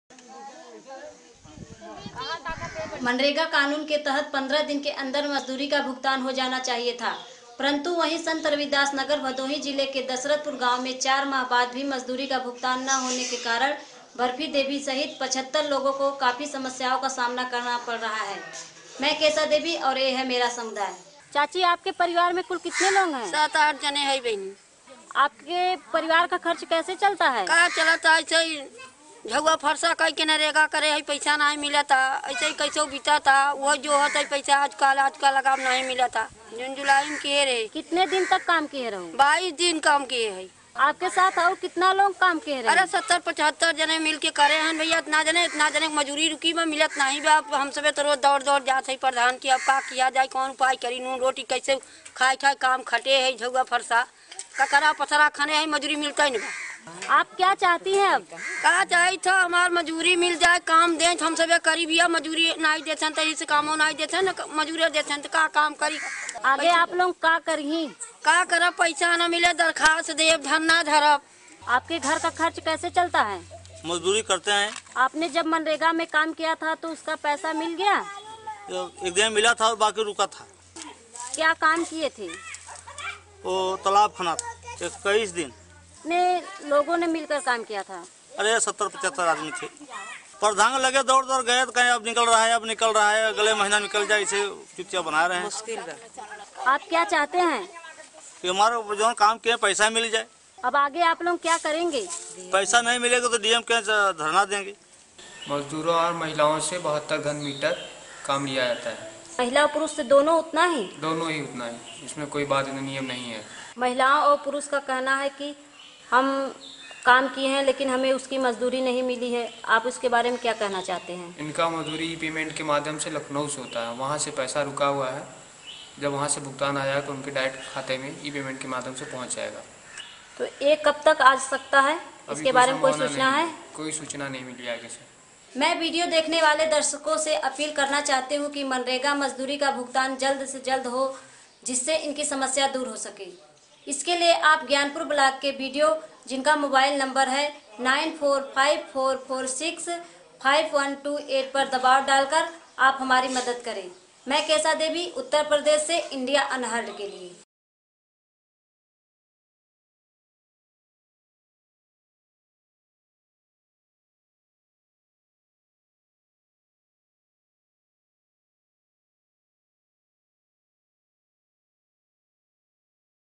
MGNREGA - No guarantee for payment
16 March 2015 | Dashrathpur, Bhadohi, UP | Kesha Devi

75 people have not yet received payments for work done 4 months ago under MGNREGA. The local authority has disconnected himself from the matter stating that the money has not yet arrived from the government itself.